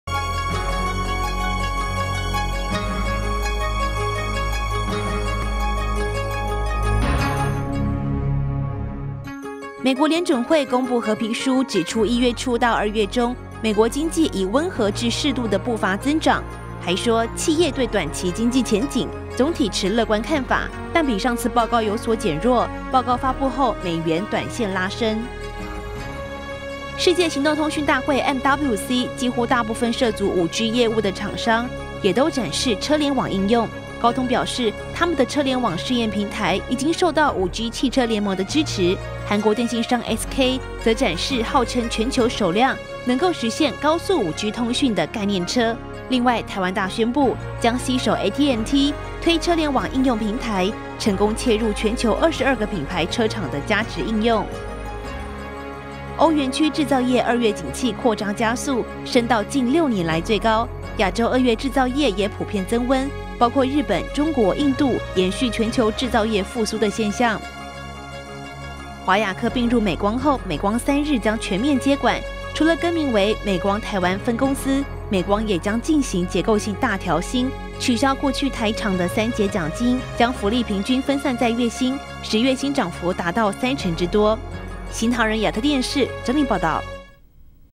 【新唐人/NTD】MWC大會 拼5G也拼車聯網｜財經100秒
FED發表褐皮書 美元短線拉升

美國聯準會公布褐皮書中，指出1月初到2月中，美國經濟以溫和至適度的步伐增長，還說，企業對短期經濟前景總體持樂觀看法，但比上次報告有所減弱。報告發布後，美元短線拉升。

MWC大會 拼5G也拼車聯網

世界行動通訊大會MWC，幾乎大部分涉足5G業務的廠商，也都展示車聯網應用。高通表示，他們的車聯網試驗平台也已受到5G汽車聯盟的支持。韓國電信商SK，則展示號稱全球首輛能夠實現高速5G通訊的概念車。另外，台灣大宣布，將攜手AT&T，推車聯網應用平台，成功切入全球22品牌車廠的加值應用。

全球製造業復甦！亞洲、歐元區PMI亮眼

歐元區製造業2月景氣擴張加速，升到近六年來最高，亞洲2月製造業普遍增溫，包括日本、中國、印度，延續全球製造業復甦的現象。

美光3日全面接管華亞科

華亞科併入美光後，美光3日將全面接管，除更名為美光台灣分公司，美光也進行結構性大調薪，取消過去台廠三節獎金，將福利平均分散在月薪，使月薪漲幅達三成之多。